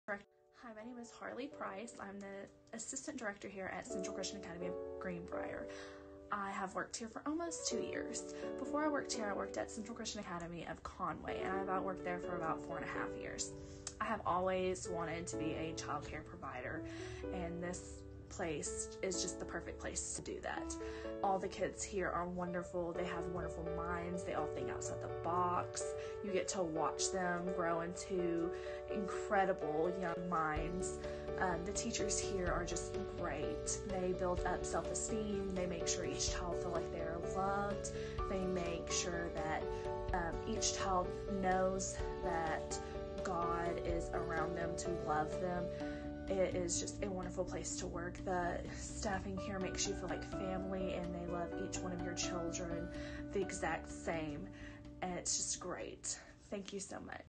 Meet the Assistant Director 😁
Watch the video to learn more about the Assistant Director  at Central Christian Academy in Greenbrier, Arkansas and why she loves working with young children! ❤

 Assis